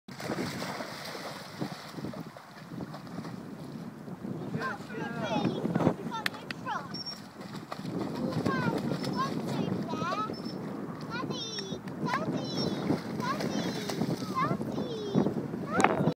Training Sky our new Chessy pup.
Our new Chesapeake bay PUP sky she loves to swim.